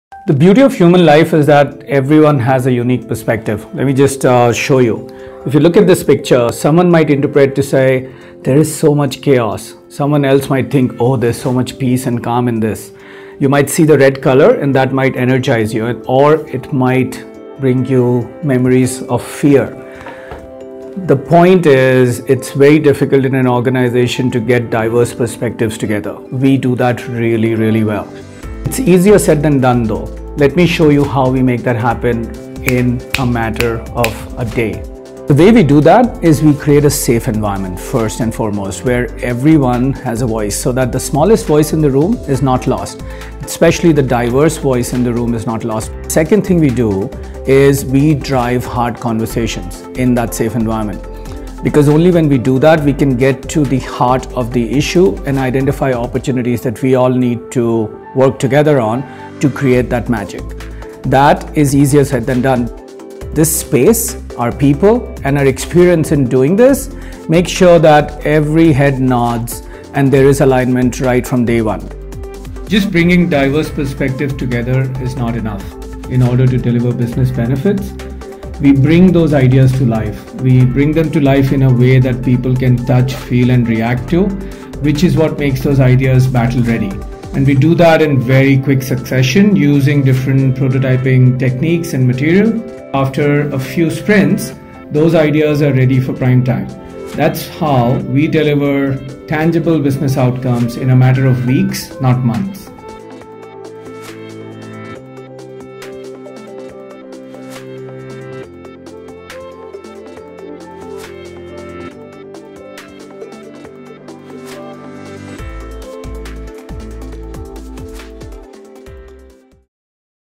Prateek Sinha on technology driven business transformation
Successful business transformations depend on how well you leverage diverse perspectives within your organisation. Watch PwC’s Prateek Sinha talk about our Experience Centres being the hub for delivering game-changing solutions.